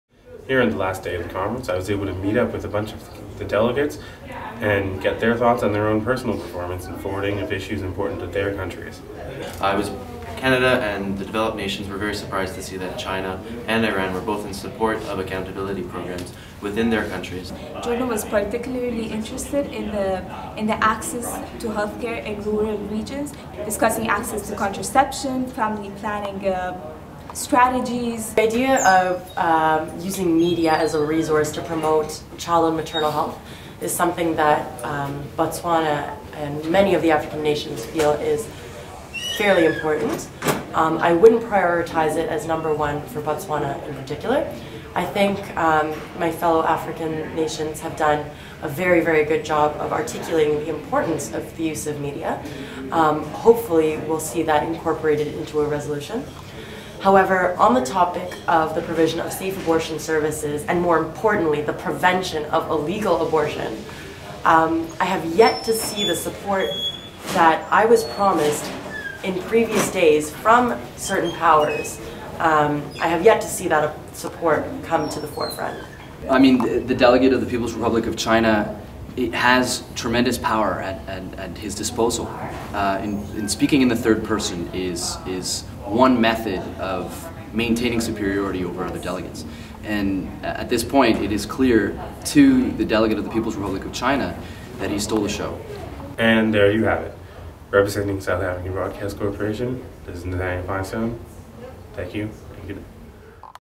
MonWHO SABC - Delegate Performance - Nathaniel Finestone
Journalist Nathaniel Finestone presents stories and analysis from the MonWHO 2011 Conference. for more information.